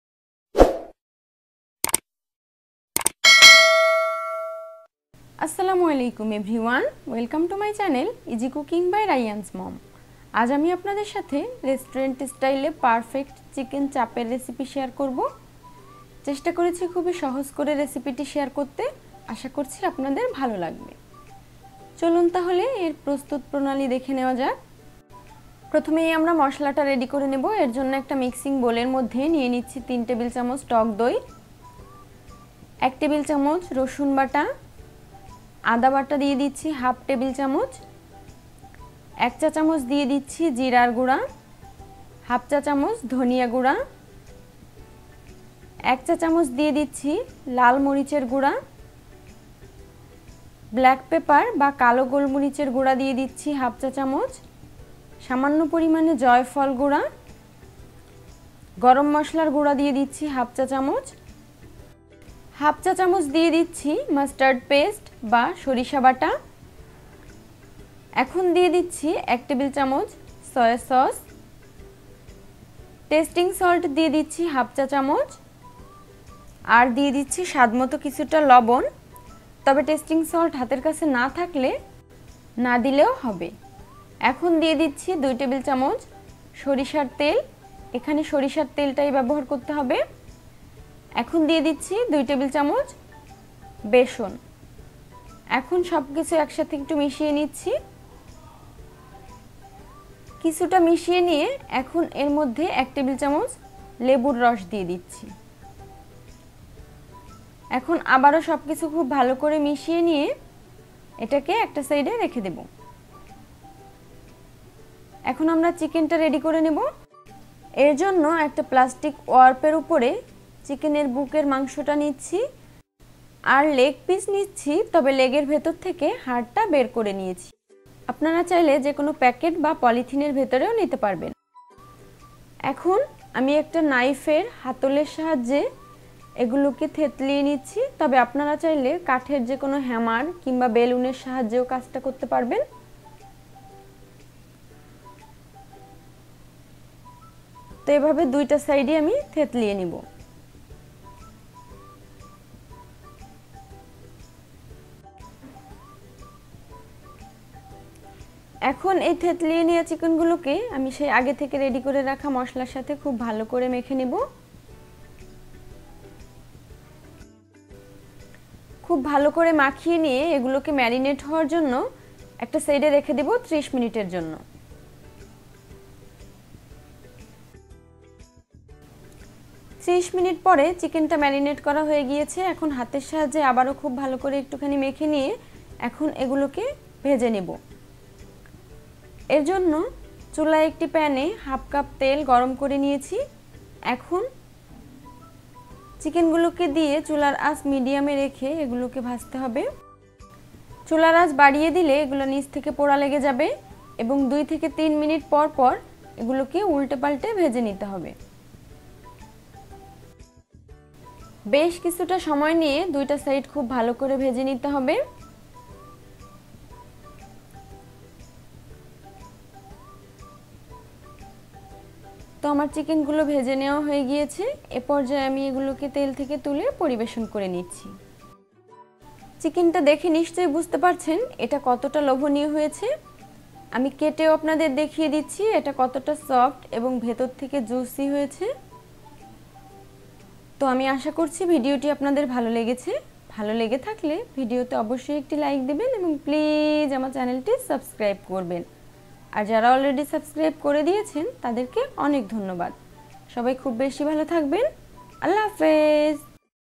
চিকেন চাপ/রেস্টুরেন্ট স্টাইলে সহজ চিকেন চাপ /Chicken Chaap/Chaap Recipe/Chicken Chaap Recipe Bangla
Ingredients :

*boneless chicken 500 mg
*sour yogurt 3 tbsp
*garlic paste 1 tbsp
*ginger paste1/2 tbsp
*black papers powder 1/2 tsp
*nutmeg powder  A pinch of
*hot spicy powder 1/2 tsp
*mustard paste 1/2 tsp
*testing salt 1/2 tsp

for fry:
soybean oil 1/2 cup